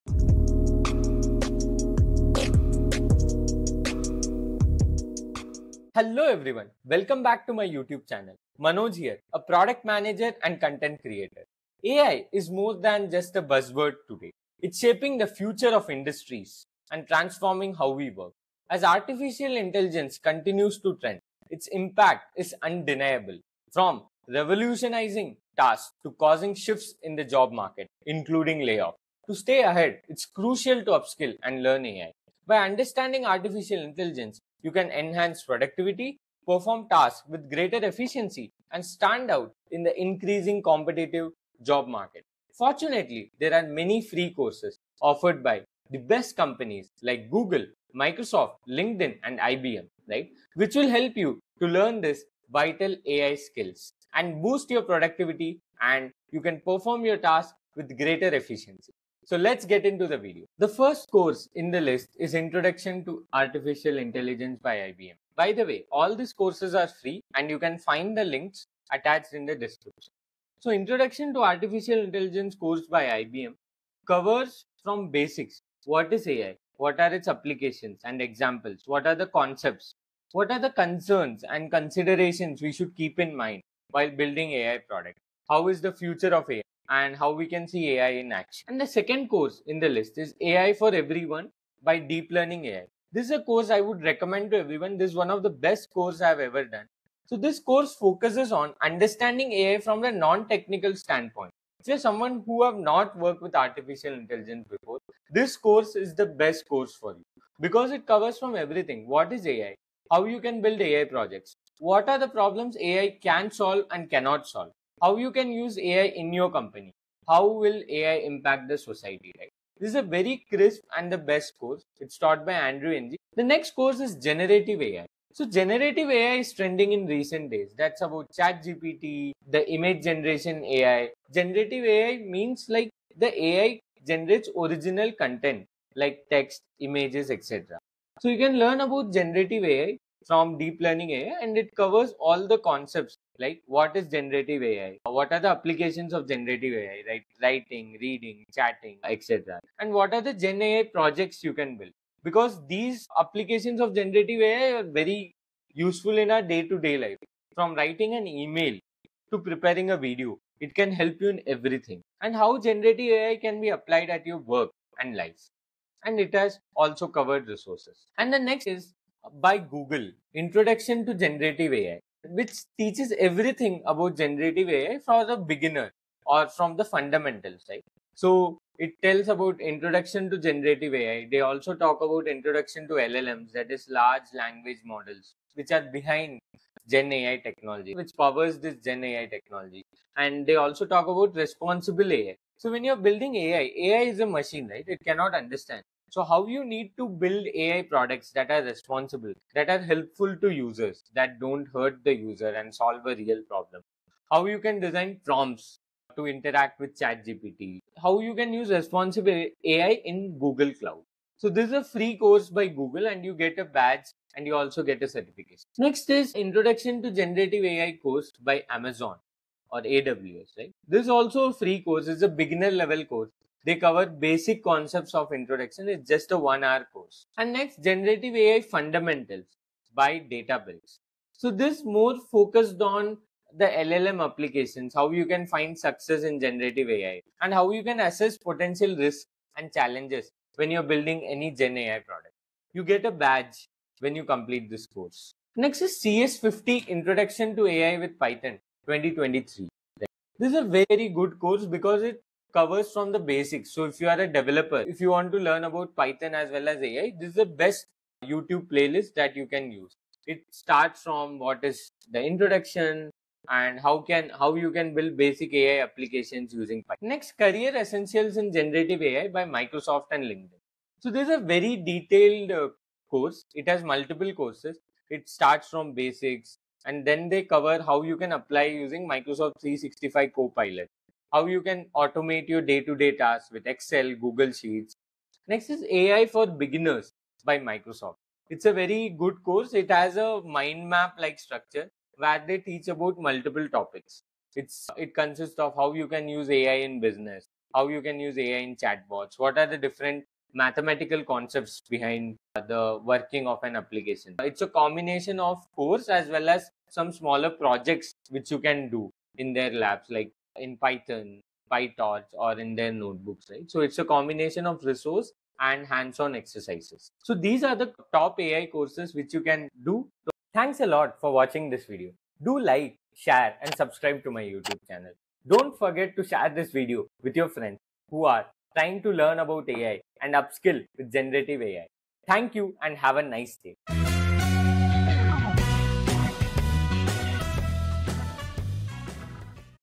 AI certifications | Artificial Intelligence free courses from Google, Microsoft | Generative AI
In this video, I talk about free courses you can use to learn about artificial intelligence and generative AI, including the technologies behind ChatGPT and Gemini. These courses are well-structured and offered by top companies like Google, Microsoft, Amazon, LinkedIn, and IBM.

The advantages of these courses are:
1) They are free.
2) You receive a certificate from top companies.
3) You learn through examples and real-life applications.
4) You can upskill yourself with AI to automate tasks, stand out in the job market, and build a high-paying career

⏰ Timestamp ⏰
Introduction: 0:00
Importance of learning AI : 0:29
Introduction to Artificial Intelligence : 1:01
AI for everyone : 1:32
Generative AI for everyone : 2:04
Introduction to Generative AI by Google : 2:50
Gen AI free course : 3:38
Generative AI fundamentals : 3:47
Introduction to AI with Python : 4:07
Career essentials in AI by Microsoft : 4:29
AI for beginners by Microsoft : 4:47
Conclusion : 5:20